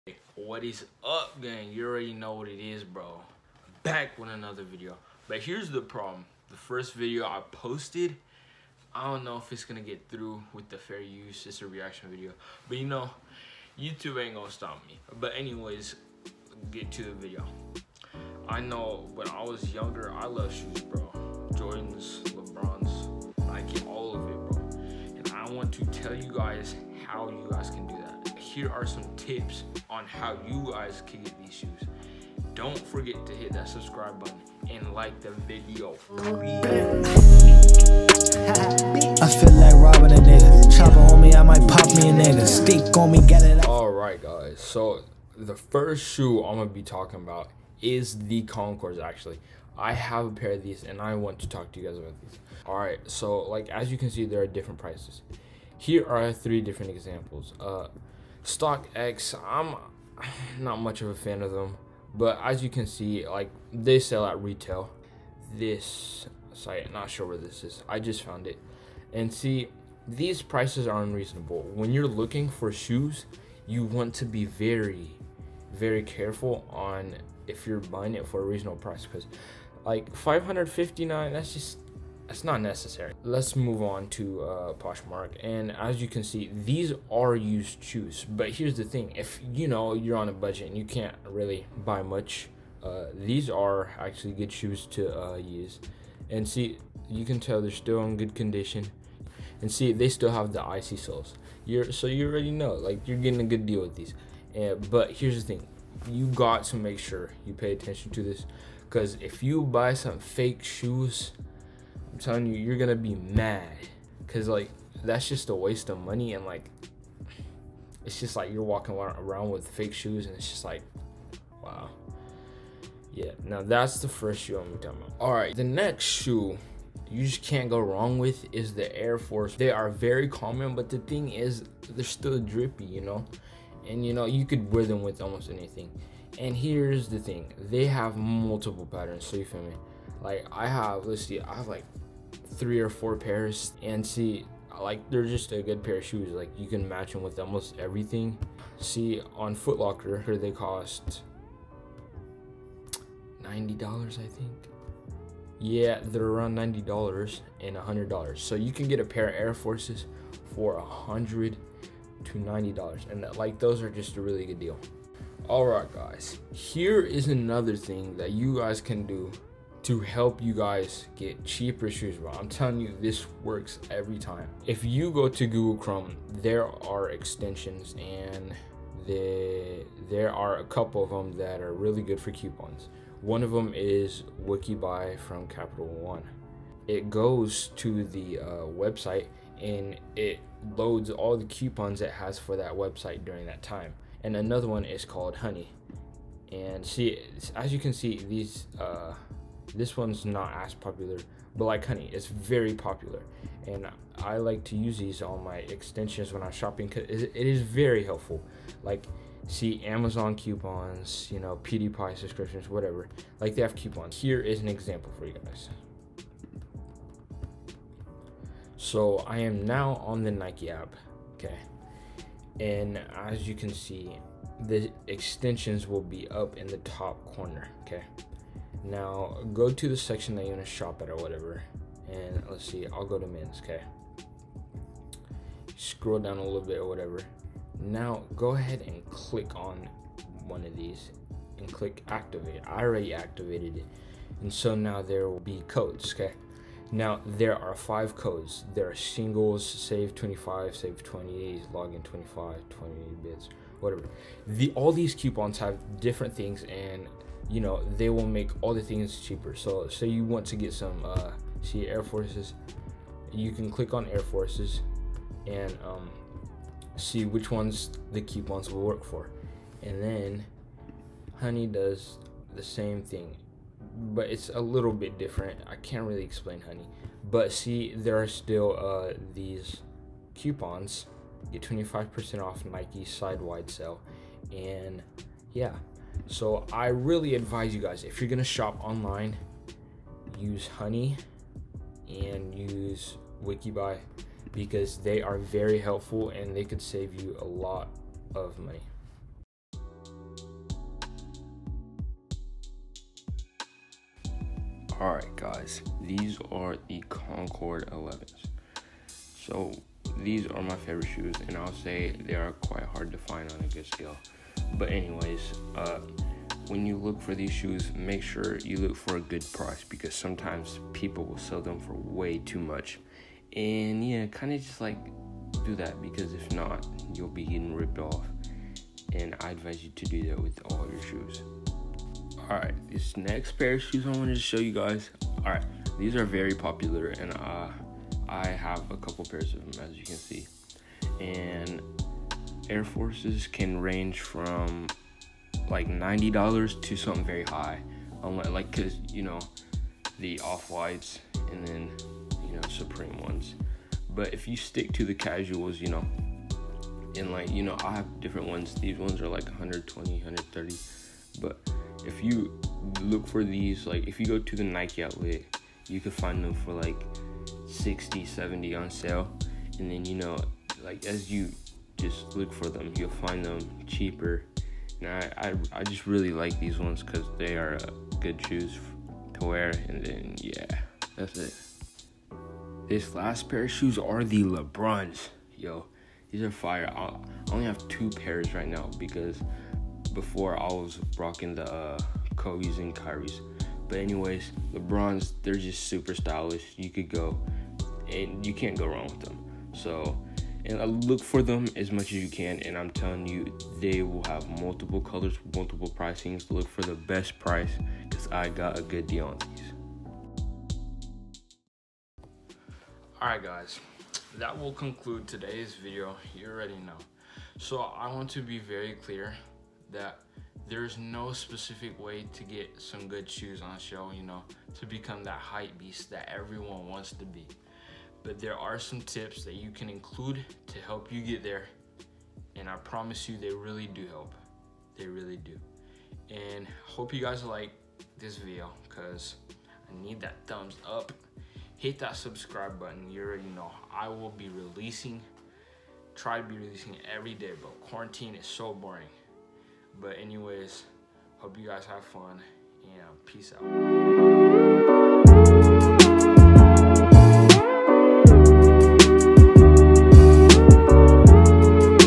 How to start a collection for shoes for cheap!! EASY TIPS
Use these tips every time you shop for shoes and you'll have dozens of shoe in no time.